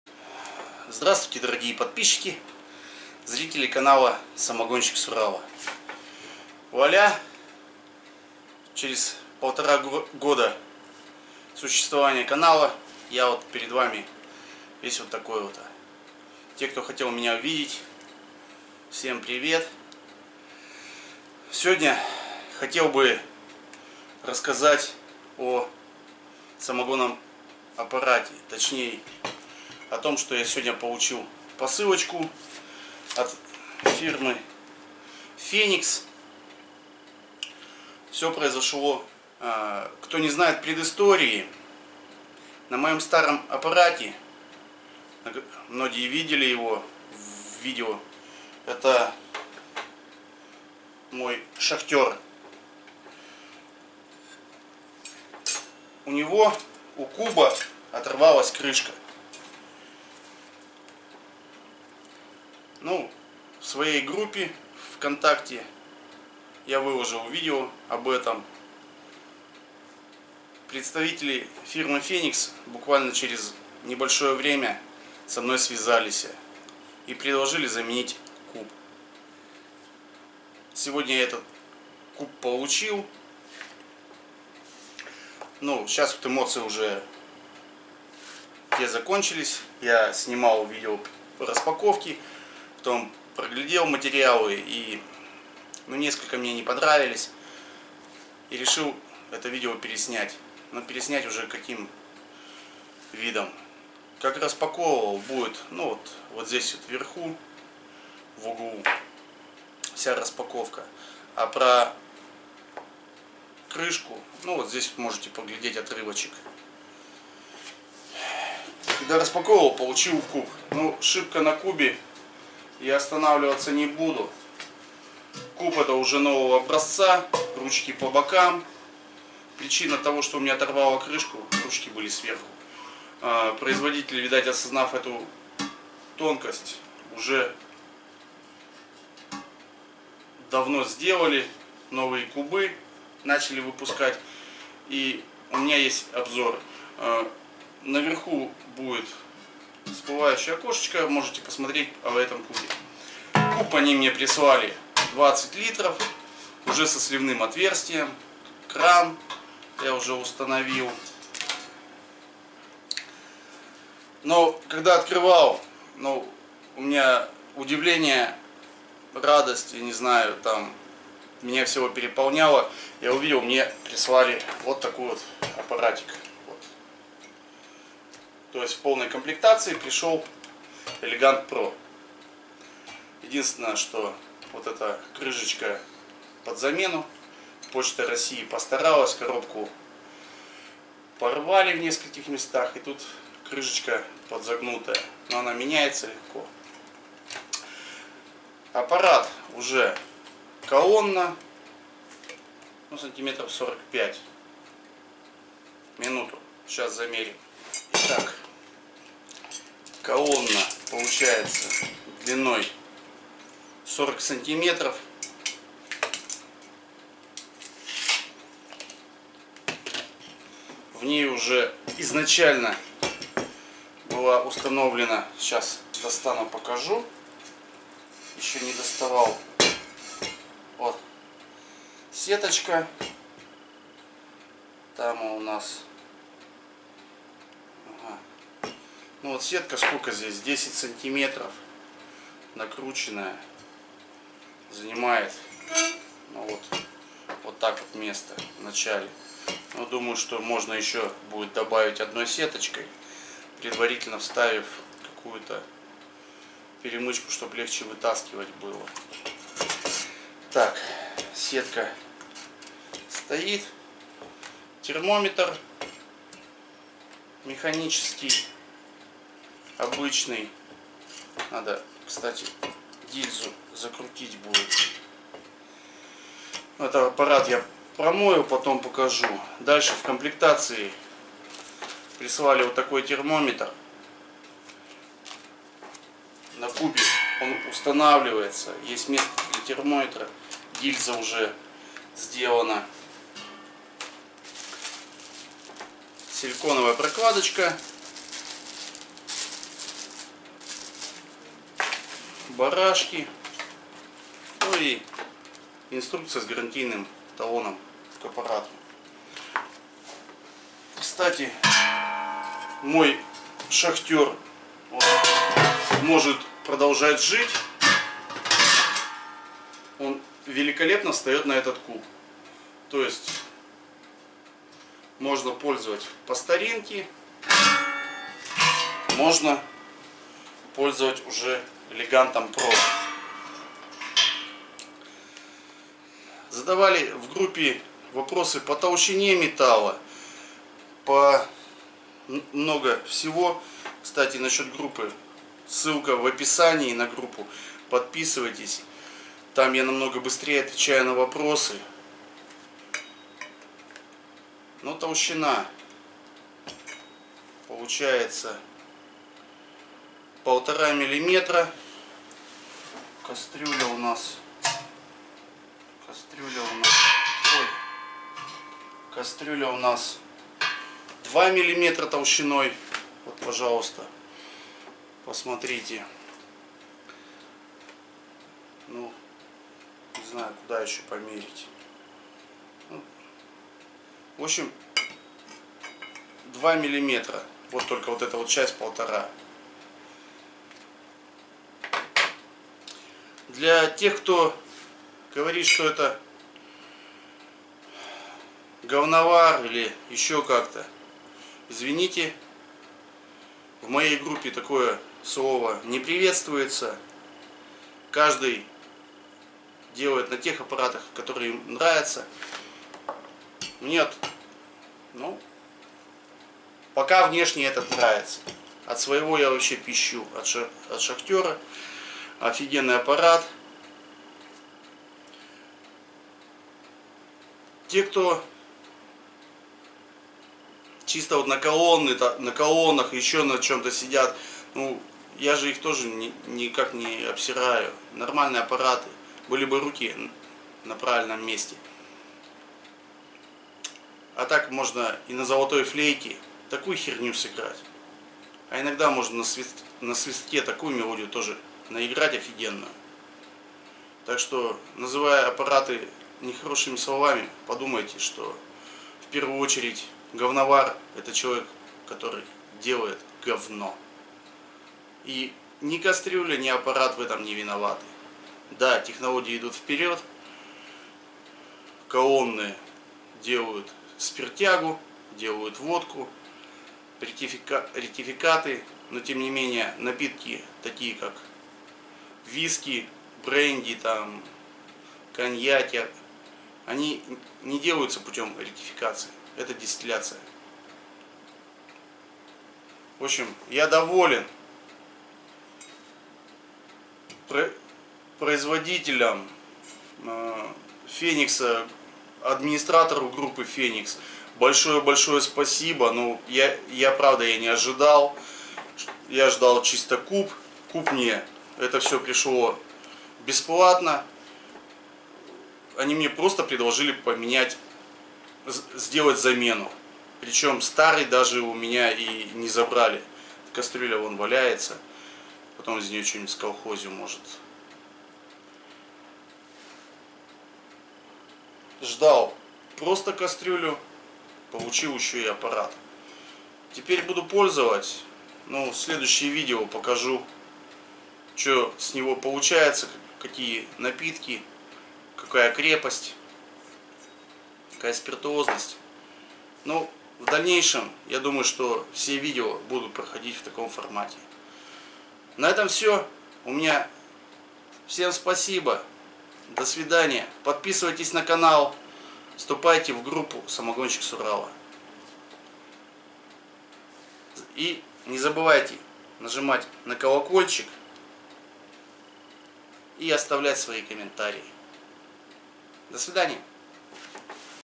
Самогонщик с Урала, посылка от Феникса, новый аппарат
Сегодня получил неожиданный подарок от компании Феникс, Прислали мне в замен моего куба, новый аппарат Элегант Экстра.
все бы так переживали за свою клеентуру. Фениксу респект.